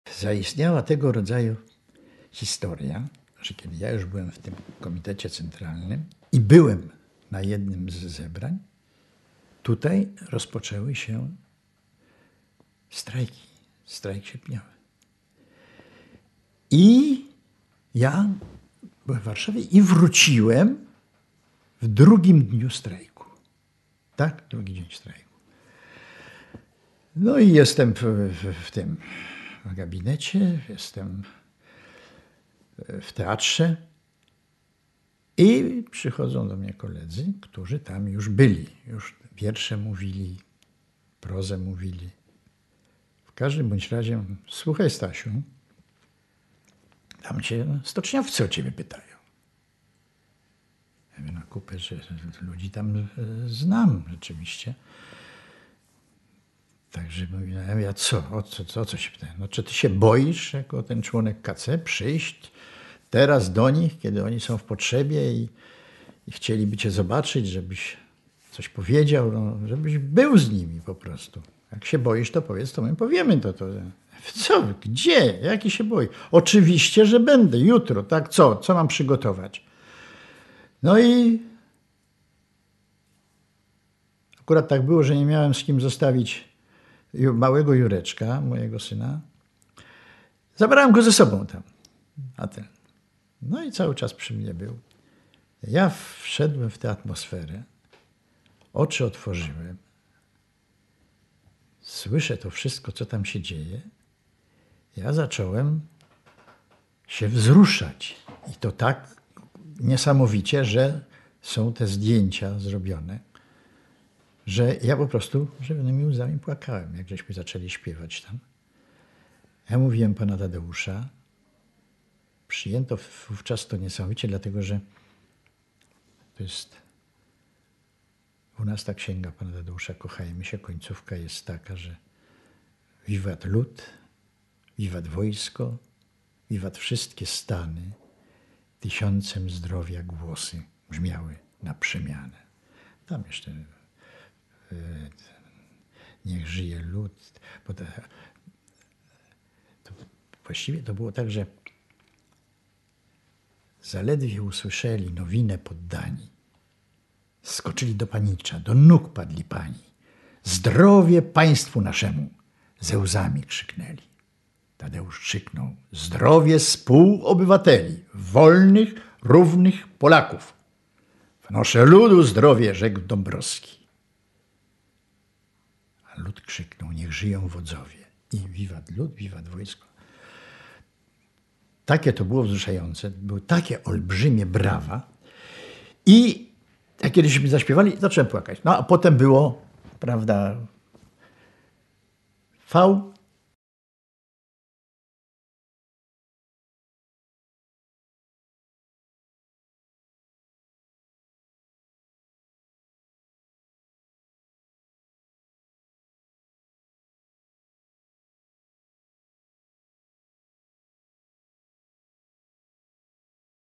Notacje ECS | Solidarność co zostało? Edycja I | Stanisław Michalski
Cykl notacji z uczestnikami ruchu społecznego Solidarność.